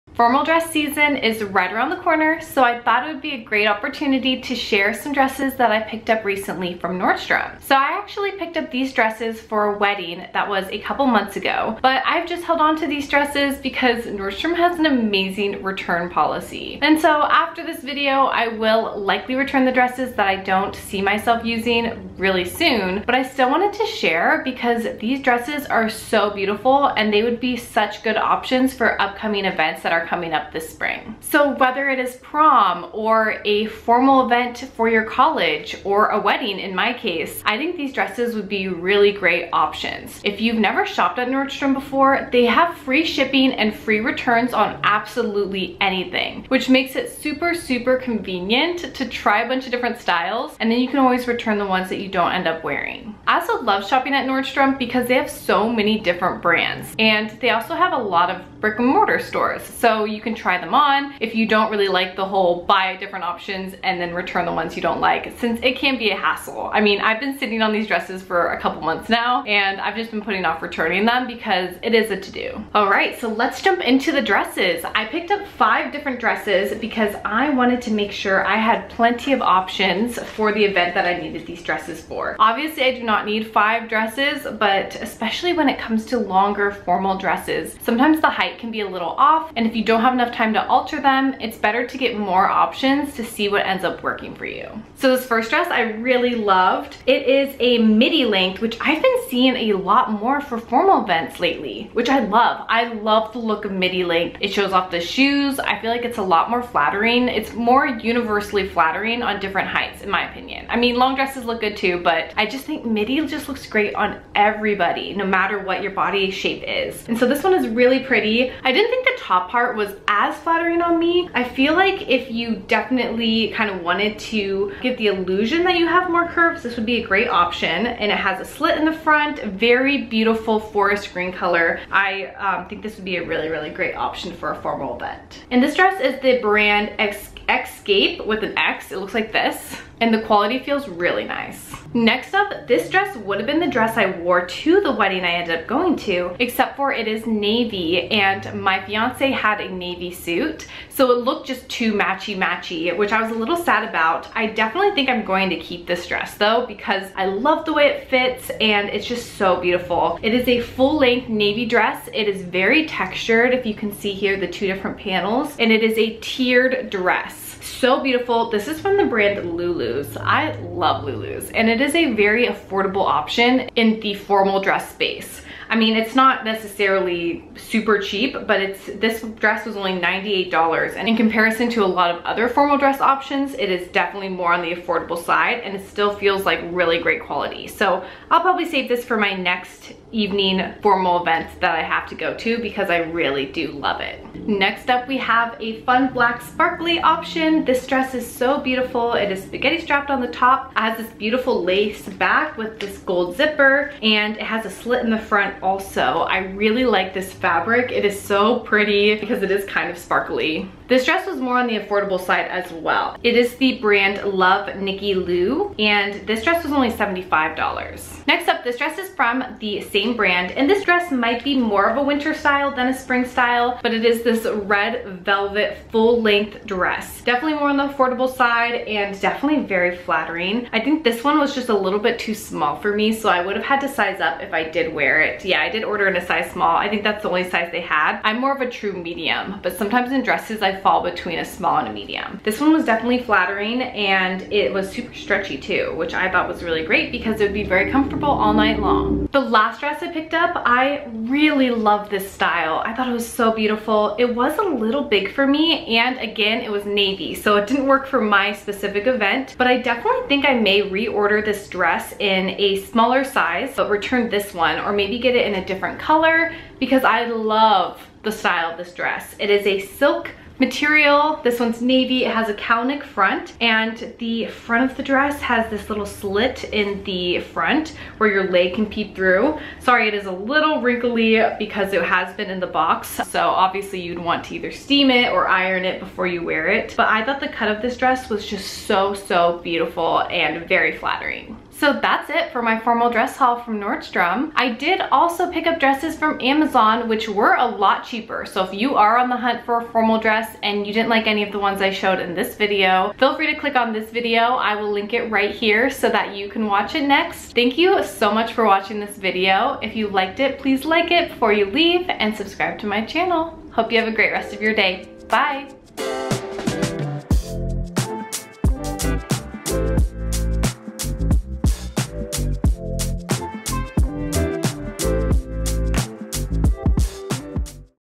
Formal Evening Dresses | Haul & Try On from Nordstrom
In this video I share a few formal evening dresses that I recently purchased for a wedding. Whether it be for a black tie evening, prom, sorority formal or any other fancy event, these dresses are perfect!